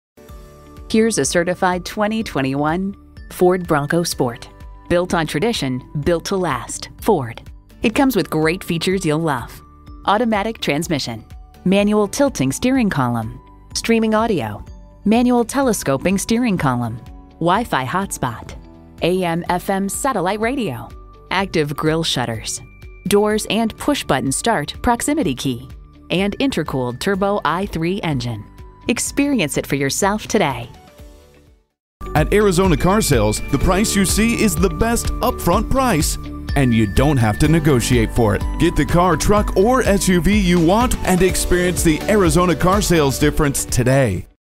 2021 Ford Bronco Sport Mesa Phoenix, AZ #21558 - SOLD
Year: 2021
Make: Ford
Model: Bronco Sport
Trim: Big Bend 4x4
Engine: 1.5 liter 3 Cylinder Engine
Transmission: 8-SPEED AUTOMATIC
Color: Alto Blue Metallic Tinted Clearcoat
Interior: Black
Mileage: 12685
Stock #: 21558
VIN: 3FMCR9B68MRA99645

Address:
1648 East Main Street